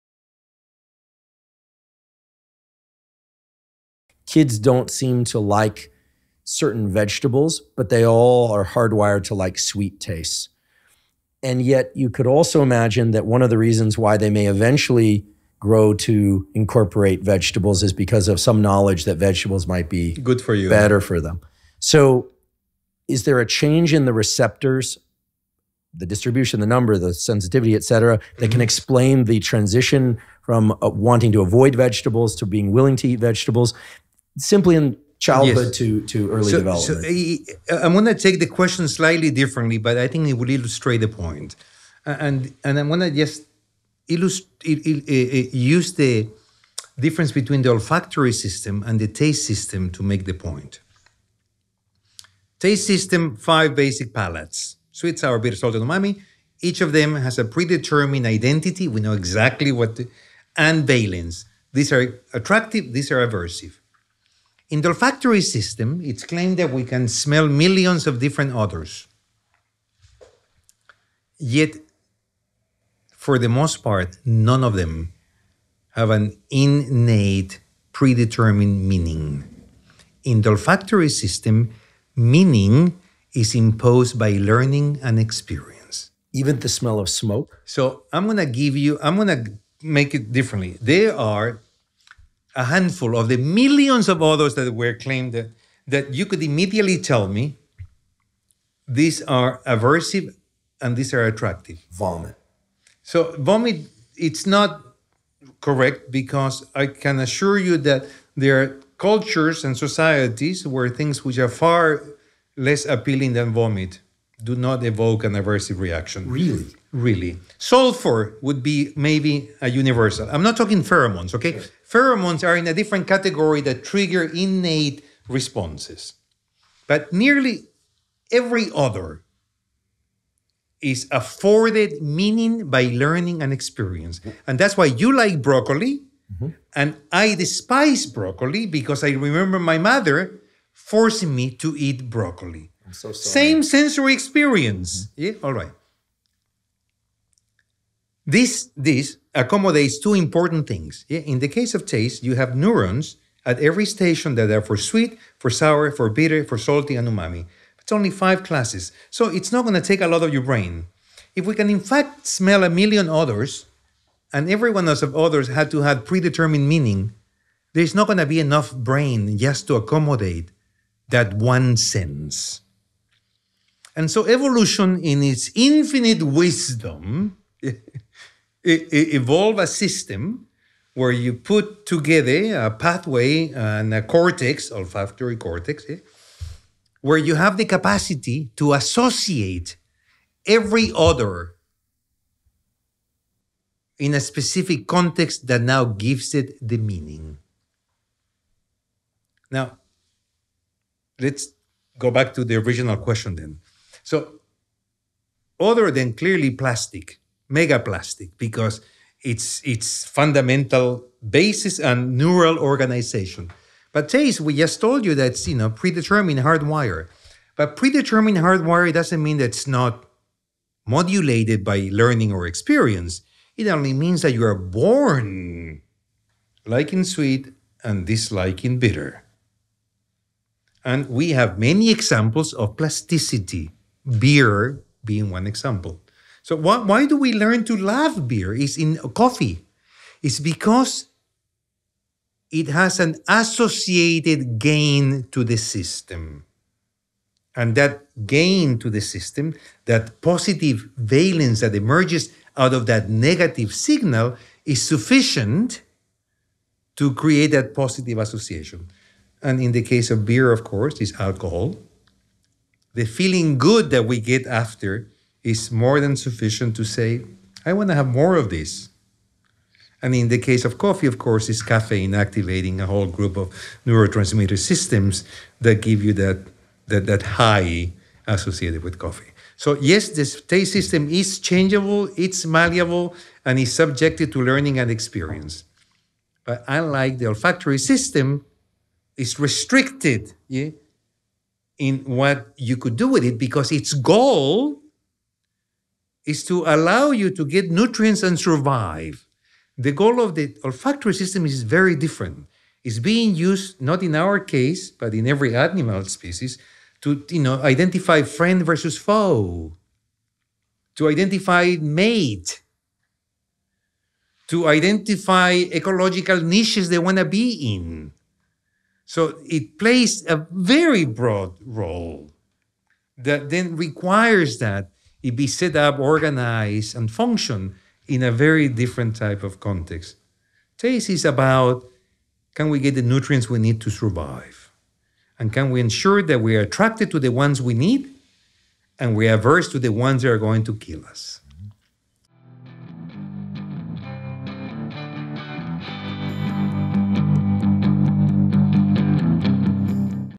Why Alcohol & Coffee Taste Bad At First & Later Taste Good | Dr. Charles Zuker & Dr. Andrew Huberman
Dr. Charles Zuker and Dr. Andrew Huberman discuss why alcohol and coffee taste bad at first, and later taste good.

Dr. Charles Zuker, PhD, Professor of Biochemistry, Molecular Biophysics and Neuroscience at Columbia University and an Investigator with the Howard Hughes Medical Institute (HHMI). Dr. Zuker is the world’s leading expert in the biology of taste, thirst and craving. Dr. Andrew Huberman is a tenured professor of neurobiology and ophthalmology at Stanford University School of Medicine and host of the Huberman Lab Podcast.